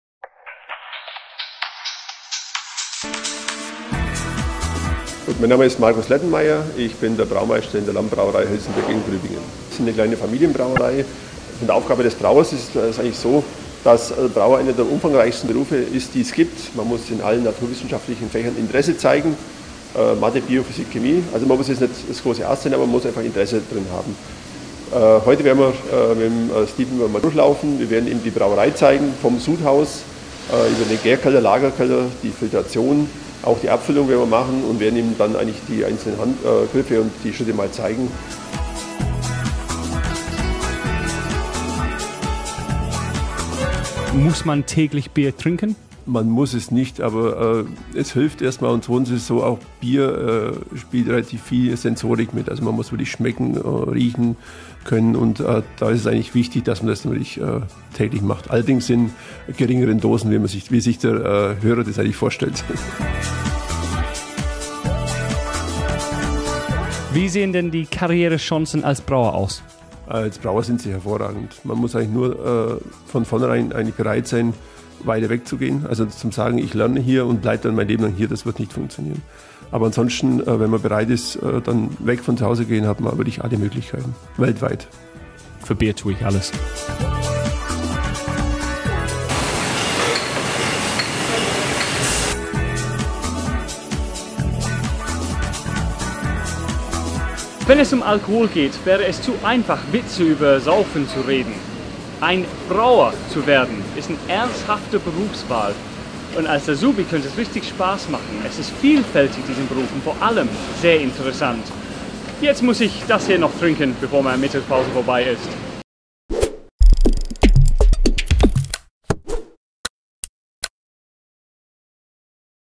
Steven trifft einen Brauer
Der DIE NEUE 107.7 Reporter Steven "The Englishman" ist zum Tag des Handwerks 2018 eine Woche lang unterwegs und darf in fünf verschiedene Handwerksberufe reinschnuppern.

Heute ging es zum Brauer und Mälzer Markus Lettenmeier in Gruibingen.